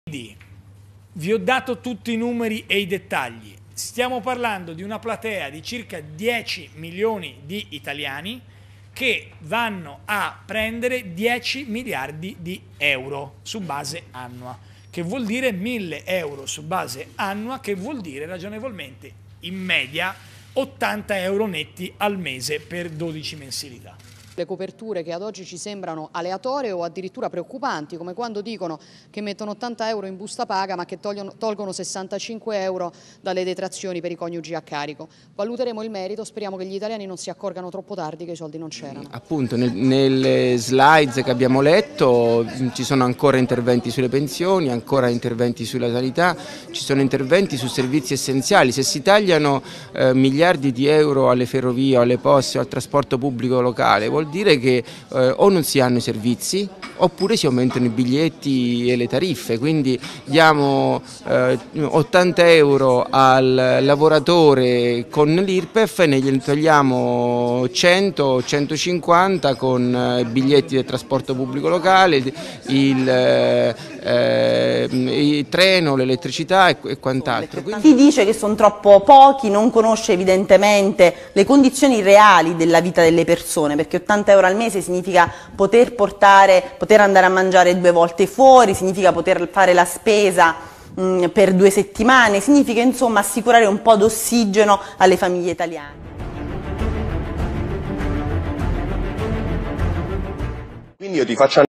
Boschi - le sue gambe belle corte
Ma chi è quella deputa-tessa che con 80 euro ci fa la spesa per due settimane - e come Padoan che non conosce quanto costa un litro di latte